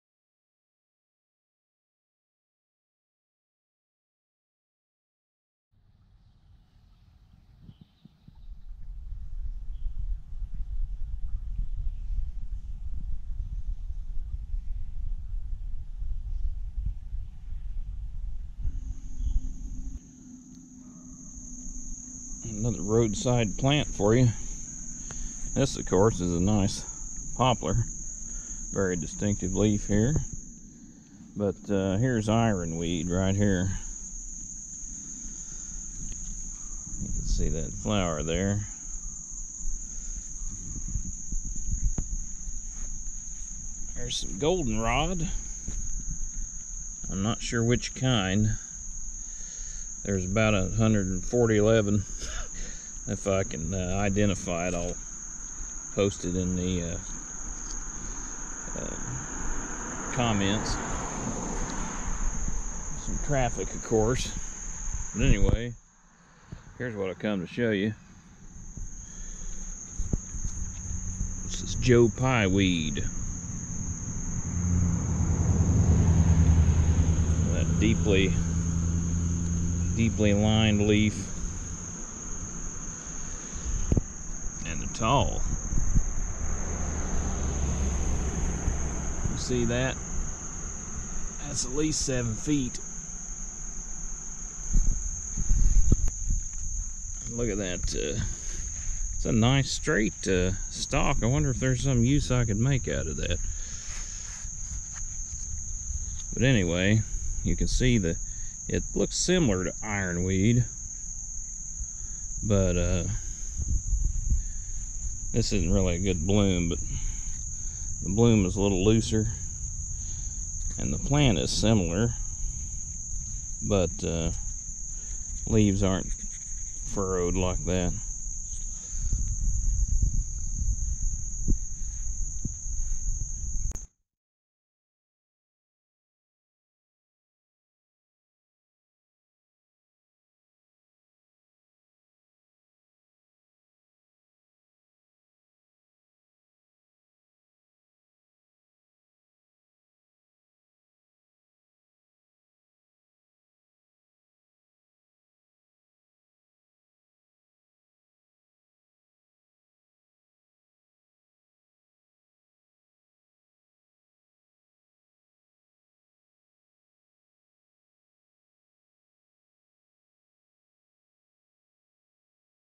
Joe Pye Weed ( Eutrochium maculatum )
Joe Pye weed ( Eutrochium maculatum ) s a North American species of flowering plant in the sunflower family Asteraceae. Widespread through much of the United States and Canada.

It can grow close to 7 ft. Stems can be purple, or green with purple spots. It produces light purple flowers in late summer and is popular with pollinators.

Joe-pyeweed loves full sun in wet areas and ditches.

It is a larval host to the Clymene moth, the eupatorium borer moth, the ruby tiger moth, and the three-lined flower moth.